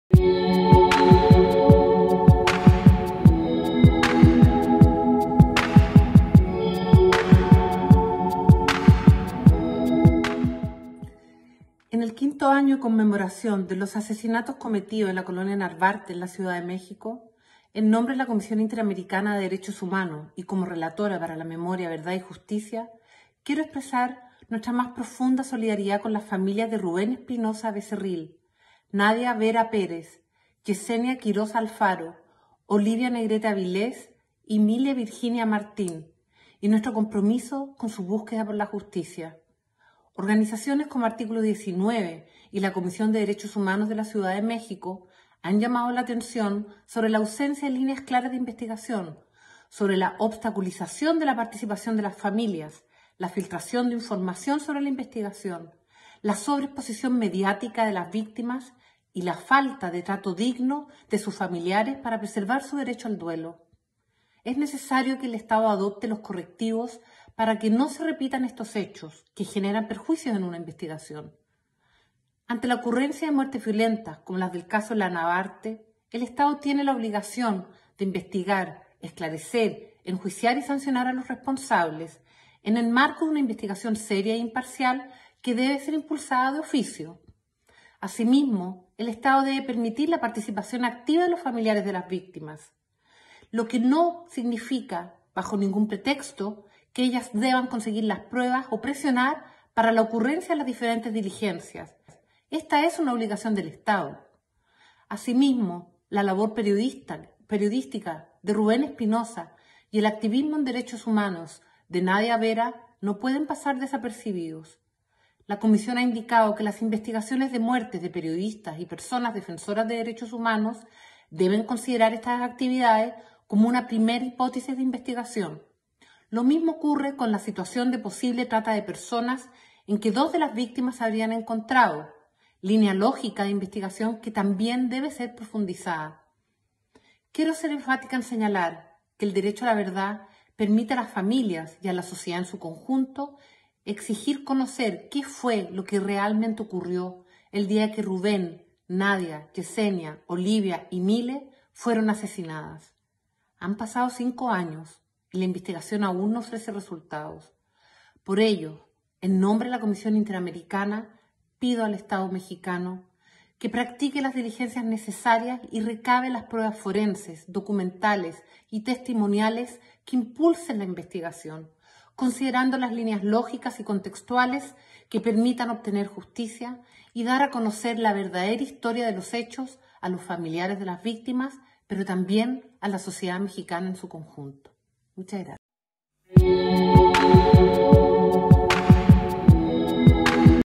Caso Narvarte: una de las deudas del Estado mexicano en materia de justicia y verdad
A 5 años de impunidad para Mile Martín, Rubén Espinosa, Alejandra Negrete, Yesenia Quiroz y Nadia Vera, como sociedad seguimos sin conocer la verdad sobre los hechos, y asímismo se ha negado el acceso a la justicia a las víctimas y sus familias.

Antonia Urrejola, relatora para la Memoria, Verdad y Justicia de la @CIDH_IACHR se pronuncia al respecto.